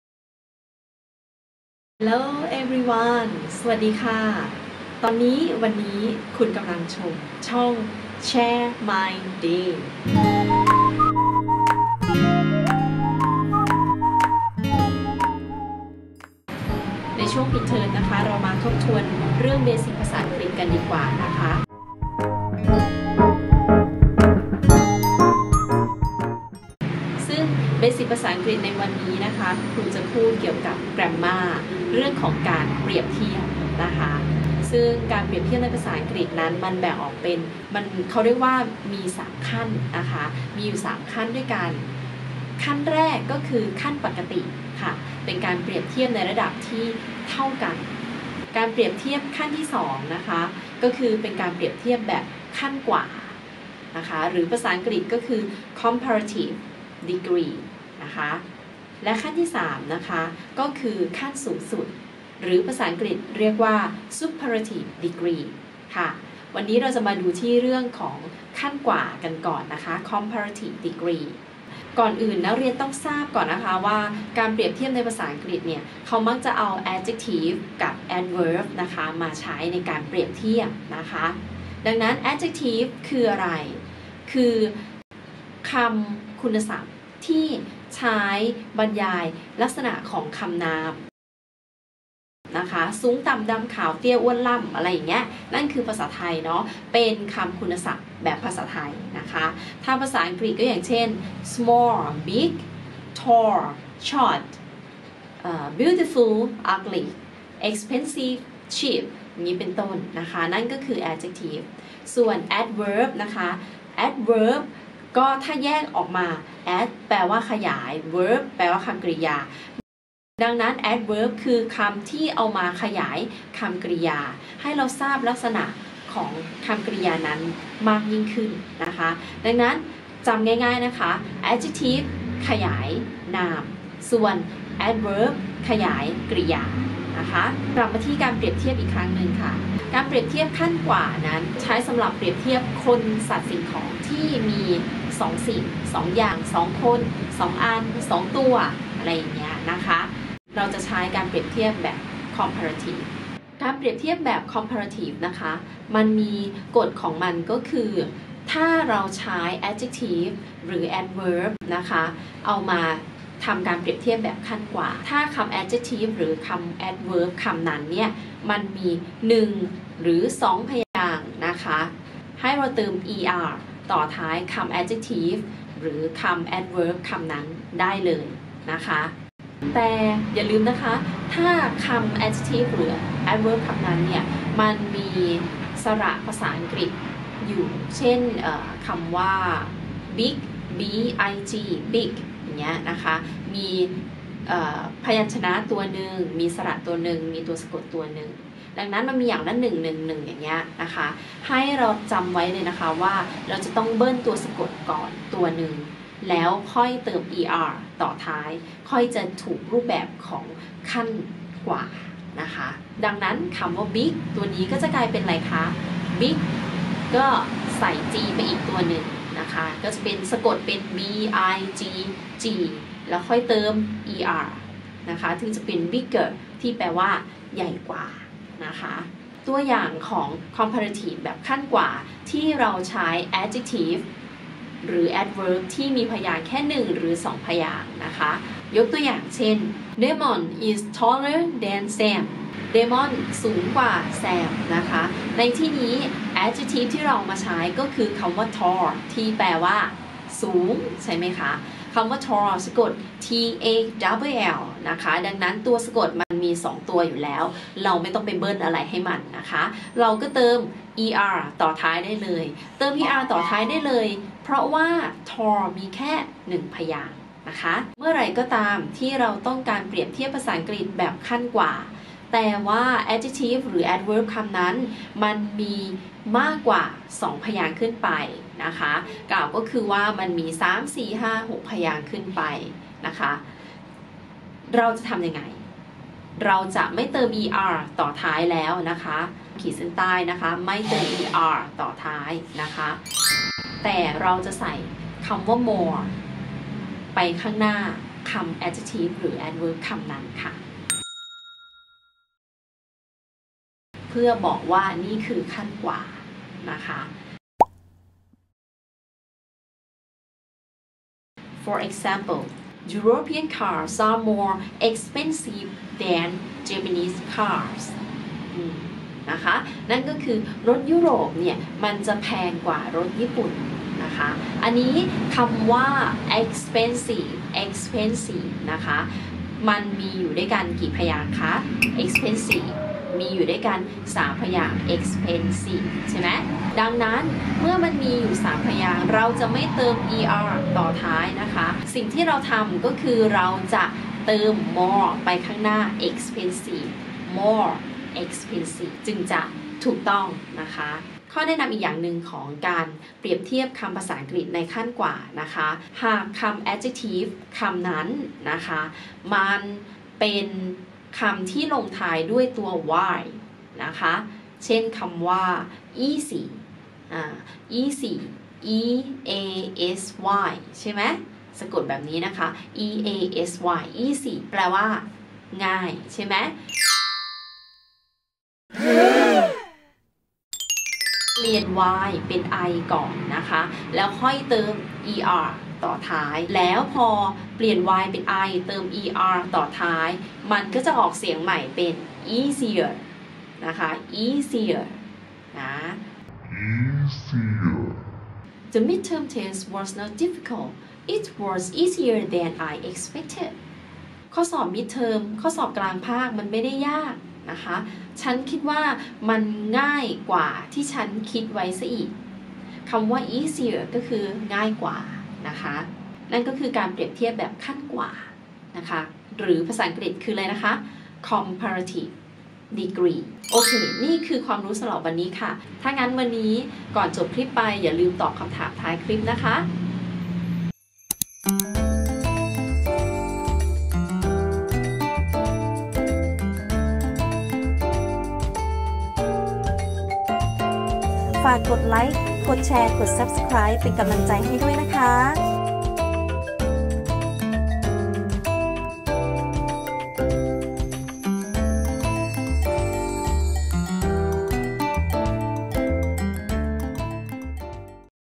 Comparative การเปรียบเทียบขั้นกว่า ใช้ยังไงให้ไม่งง l เรียนภาษาอังกฤษ ยุคโควิดระบาด l ShareMyDay
สวัสดีจ้า 🍀 ใครกำลังงงเรื่อการเปรียบเทียบในภาษาอังกฤษกันจ้ะ
รู้ไหมการเปรียบเทียบแบ่งเป็น 3 ขั้นนะจ้ะ มีอะไบ้างไปเรียนด้วยกันเลยจ้า

ฝากติดตามเพจและช่องด้วยนะจ้ะ
ใครชอบคลิปสอนภาษาอังกฤษก็กด Like กด share ให้กำลังใจกันด้วยนะจ้ะ
ใครอยากให้ทำคลิปอะไรก็ comments บอกกันได้นะจ้ะ
Hope you enjoy learning English 📚